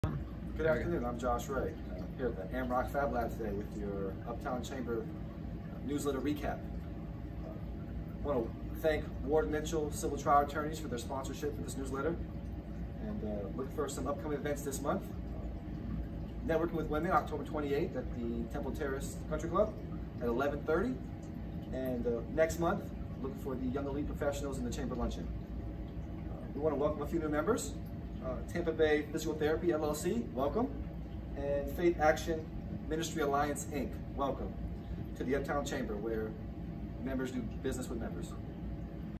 Uptown Chamber - NEWSLETTER HIGHLIGHTS - October 22, 2021
Josh Ray of Grow2Greatness Ventures recaps what you may have missed in last week's Uptown Chamber email newsletter!

-- NEW MEMBERS --

Faith Action Ministry Alliance, Inc.